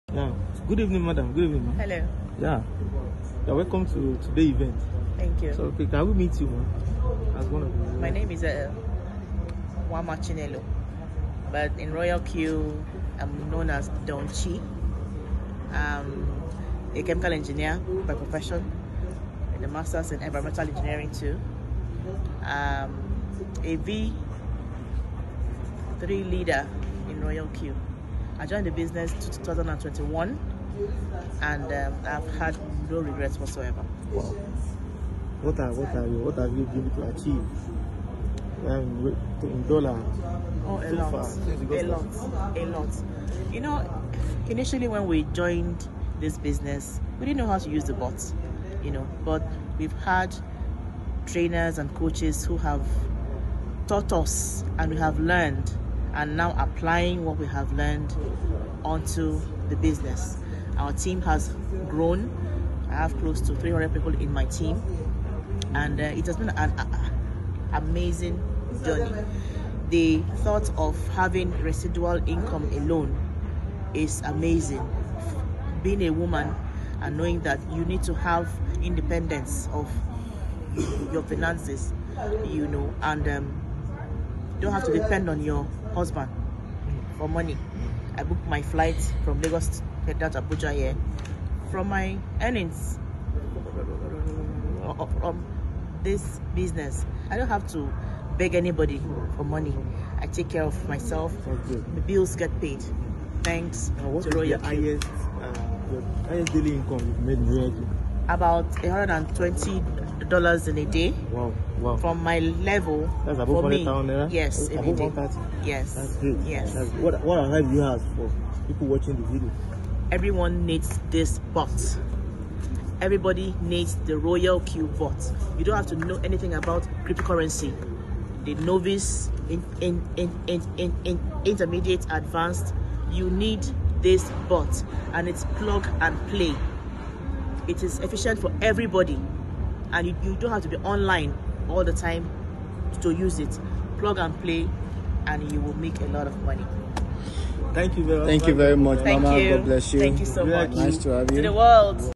Royal Q Testimony - She Earns Daily $100 Plus
Royal Q Is A Quantitative Bot.

It helps you to buy and sell and also drop your profit in your Binance account.

In Royal Q you don't give anyone your money. You monitor it yourself.

Stop giving your hard-earned money to scam projects, please. Watch the video below for understanding